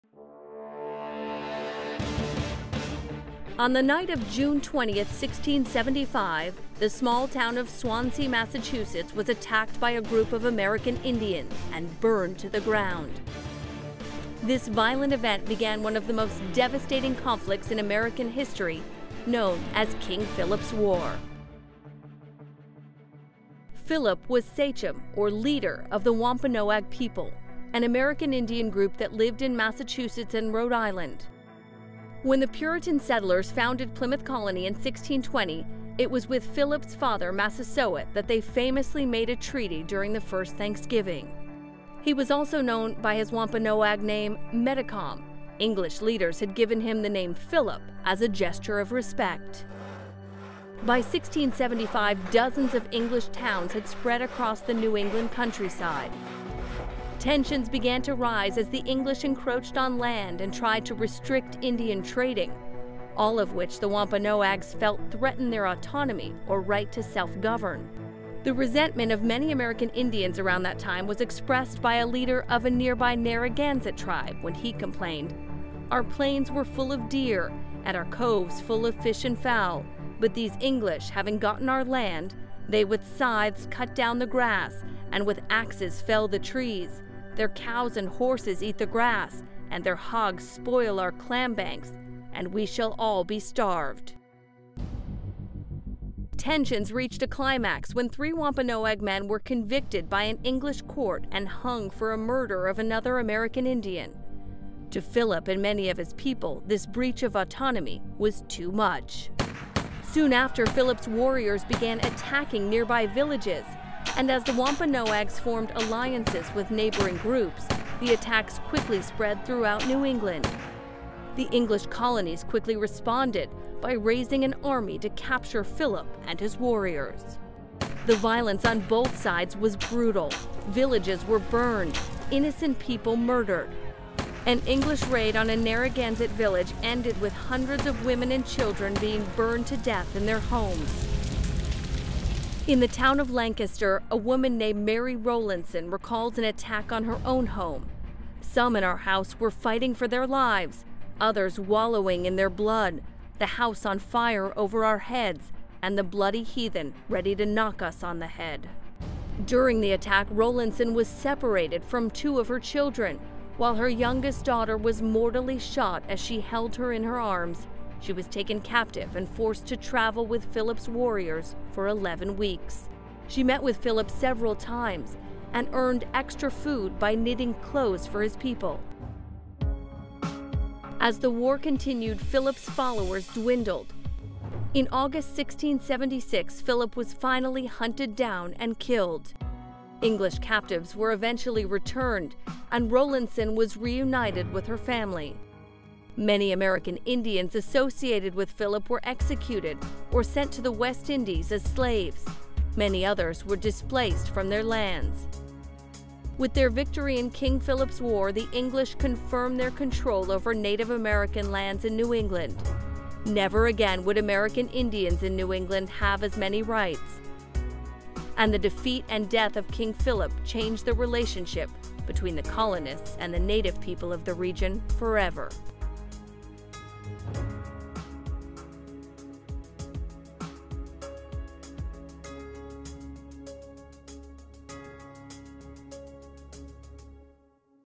King Philip and Mary Rowlandson
In 1675, an American Indian by the name of King Philip launched an attack on an English village, triggering a a full scale war that ravaged the New England countryside. Massachusetts settler Mary Rowlandson was taken captive during the war and forced to travel with Philip's warriors for 11 weeks.